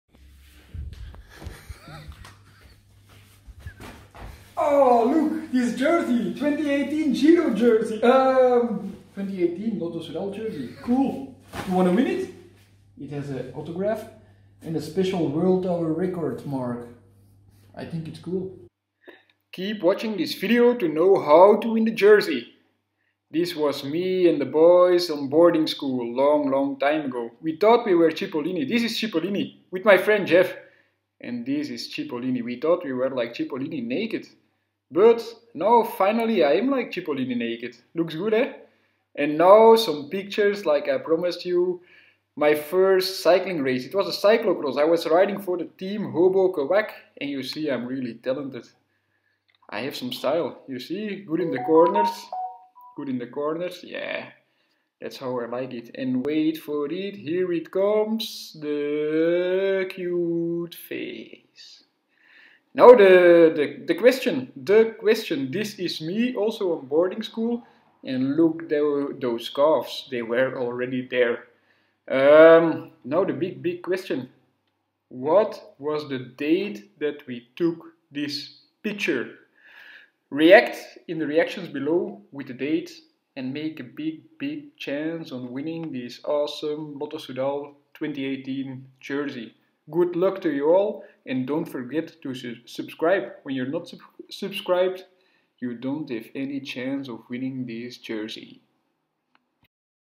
ANOTHER GIVEAWAY
Check et ut, hasten!

Send in your answer in reaction below BEFORE Tuesday 7th of April.
If no one guesses the right date, we pick the closest.
If more than one guesses right, the first one who guessed right, wins.

good luck

General:
will be disqualified.
-Youtube is not a sponsor of this giveaway, nor bears any liability for this contest
-We will only use personal data of the winner to get in contact through social media. If this does not work, we will post a video with the winners name to get in contact with @Jefvanm on instagram.
-The price question is in the vlog, entries are made by reacting in the section below. Price winners will be announced within at least 10 days after the competition started, unless otherwise stated.